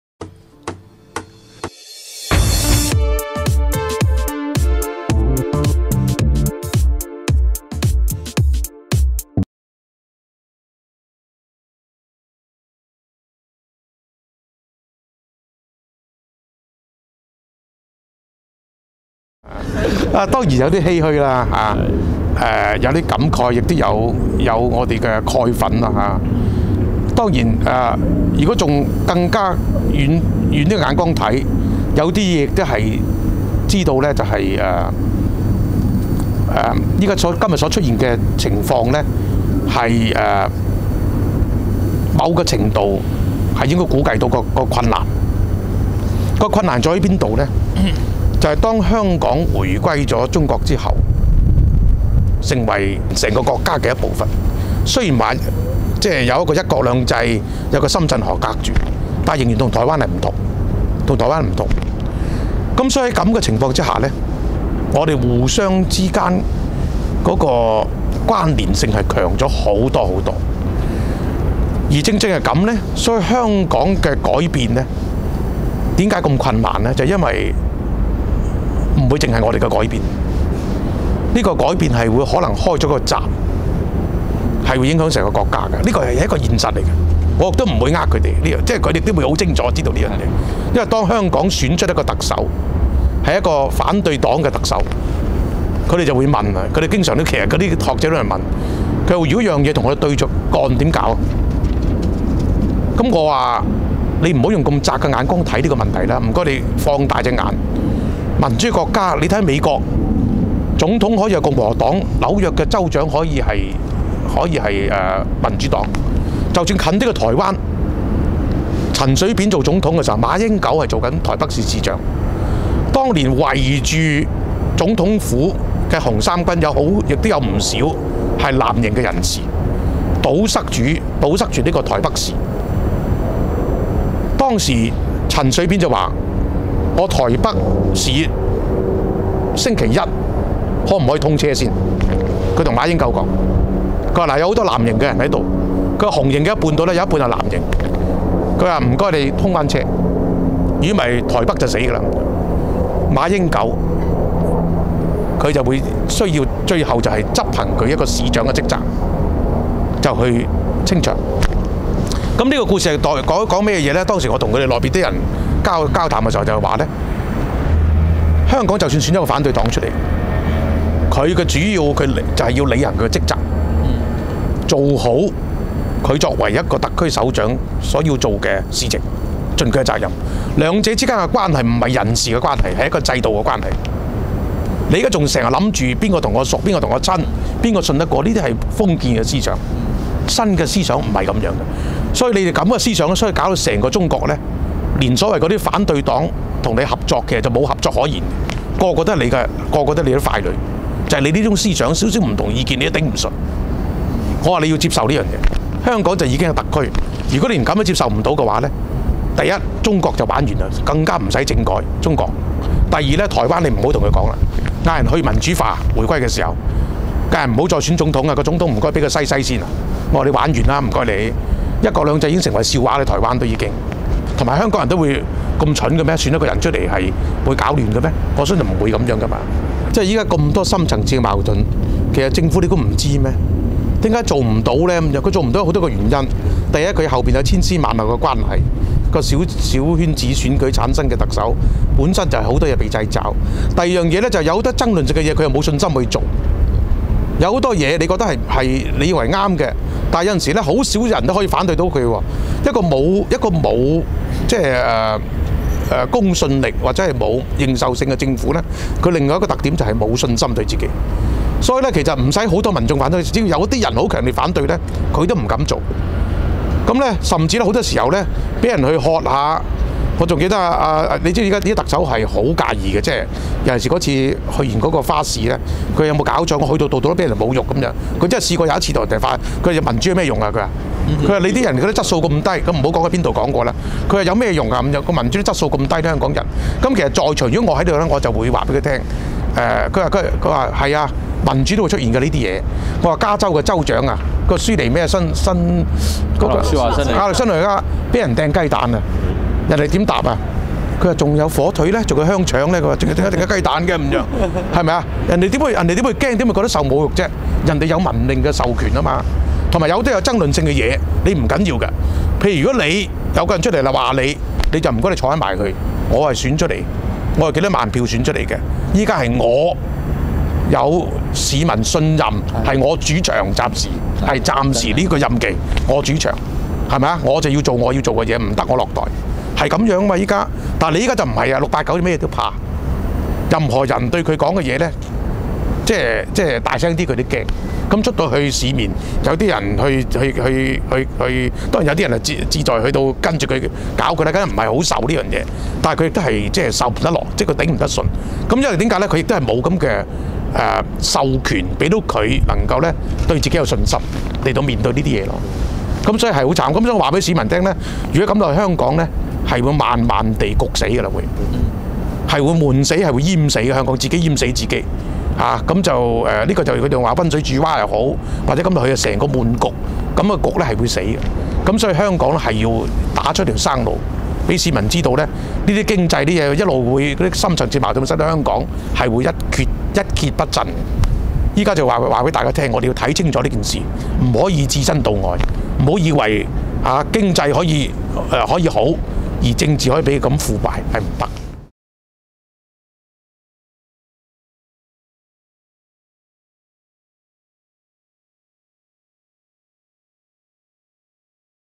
何俊仁：一國兩制已成為笑話
何俊仁歎道，早料到民主路的困難，「香港跟台灣不同，香港跟中國互相的關連性強得多，北京始終擔心開咗個閘，會影響成個國家，擔心有反對黨做特首。」惟他仍質疑，北京的想法封建，「美國同紐約州，又或者陳水扁當總統那時候跟台北市長馬英九，就算不同政黨，兩者間都會履行職責，有制度上的關連」，中央擔心反對派當特首，令一國兩制成為笑話